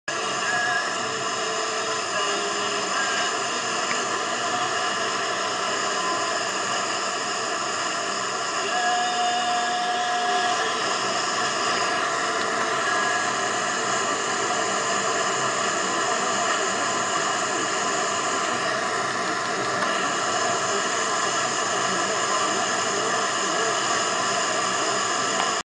FM DX: Sporadic-E Reception of UNID Quran Palestine on 88.9 in Pyrgos, SW Greece
A UNID Arabic religious station received in Greece during E-Skip above south Turkey. Possibly from Syria, Lebanon or Jordan. Any ideas?
QTH: Pyrgos, SW Greece
RX: JVC MX-J150R + indoor whip antenna
Received at 2.6.2013